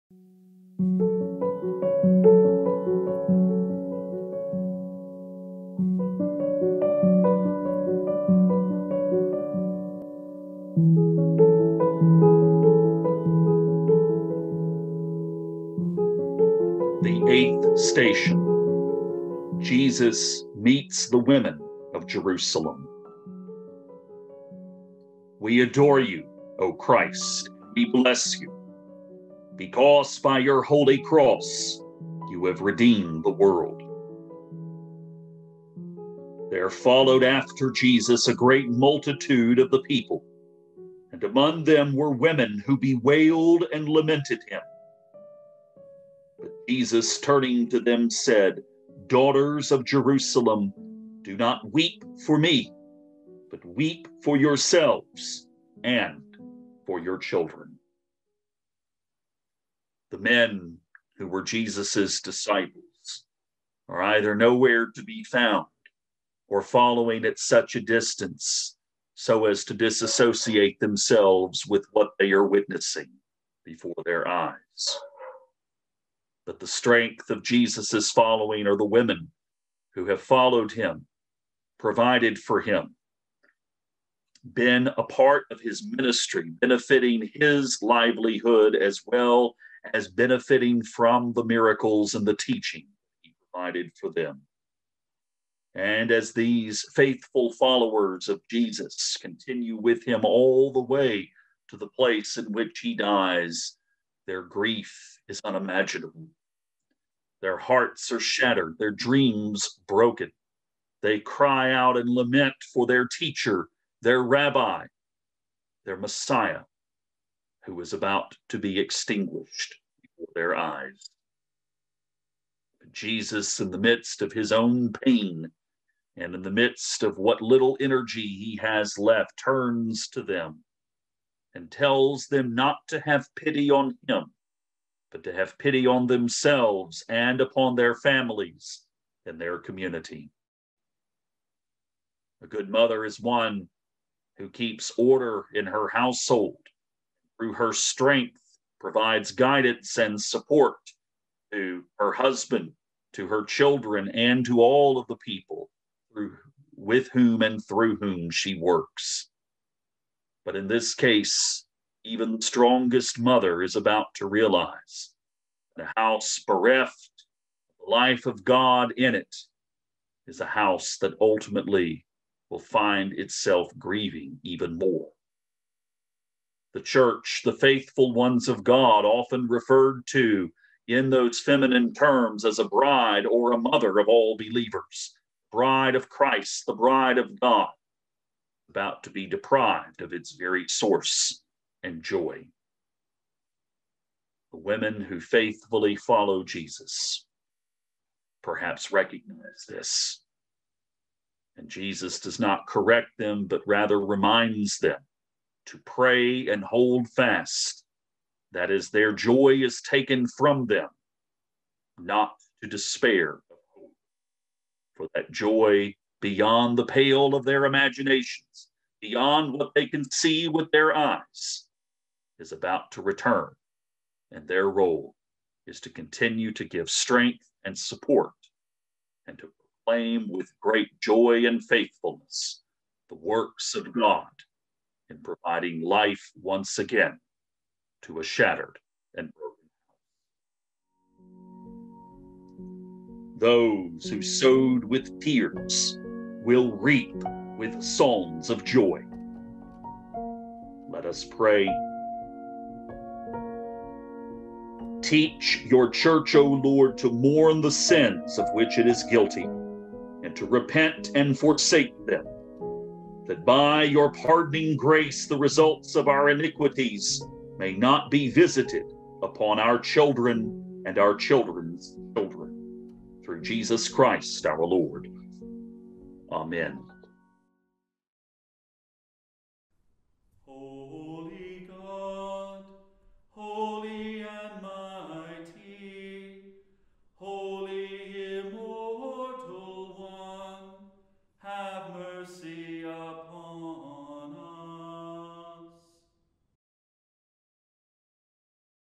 Holy Week 2021: Stations of the Cross: The Eighth Station: Jesus meets the women of Jerusalem.
Led by Fr. Bailey Norman, Priest-in-Charge, St. Stephen's Episcopal Church, Oak Ridge, Tennessee.

Artwork: Stations of the Cross, from Church of the Holy Trinity in Gemunden am Main, Bavaria, Germany.